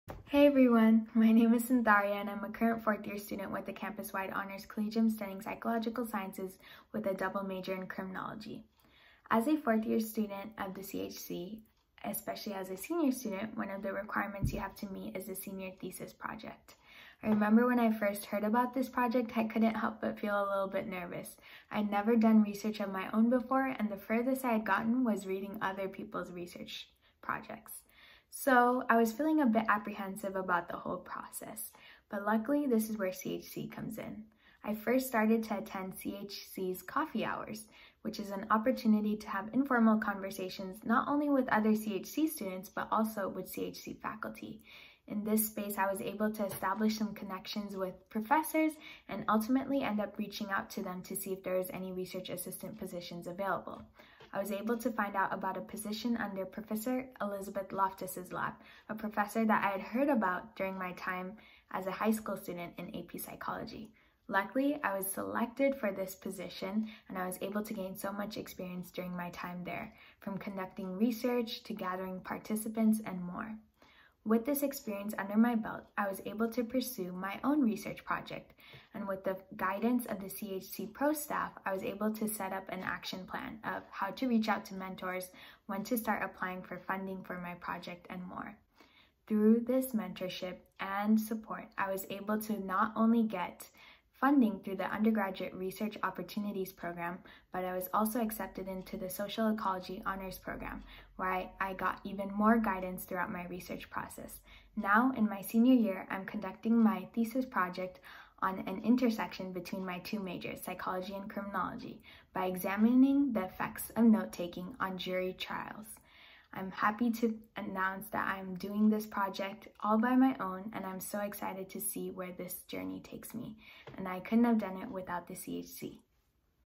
How CHC Helped Me Launch My Research Project
Campuswide Honors Collegium Ambassador Sauntharya shares about honors research.  Learn more about honors research at honors.uci.edu.